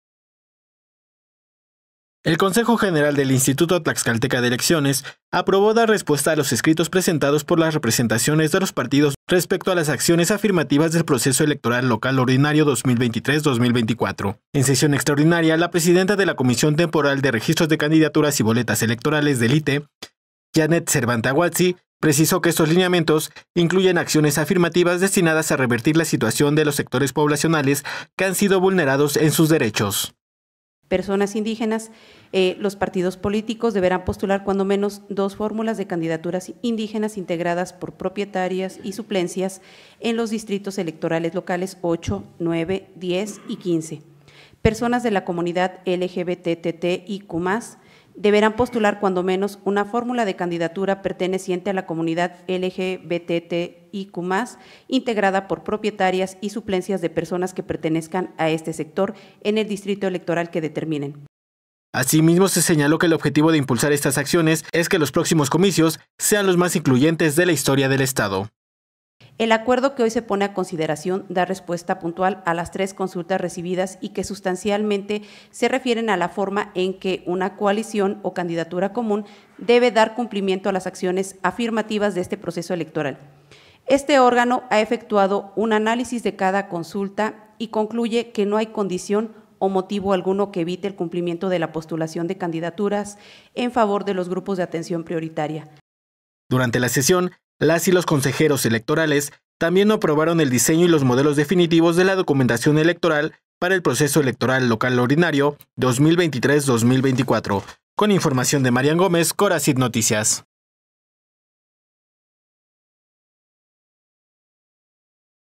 Realiza ITE sesión extraordinaria
El Consejo General aprobó el diseño y modelos definitivos de la documentación electoral para el proceso electoral local ordinario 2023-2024.